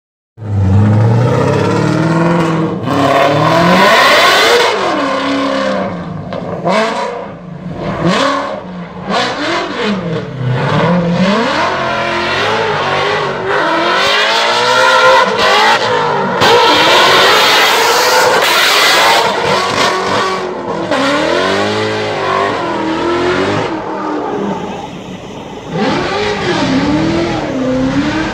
Justin Pawlak taps the wall & destroys bumper
Justin Pawlak at the Stifflers Fun Runs at Mustang Week 2016 showing off for the crowd and taps the wall taking out his rear bumper.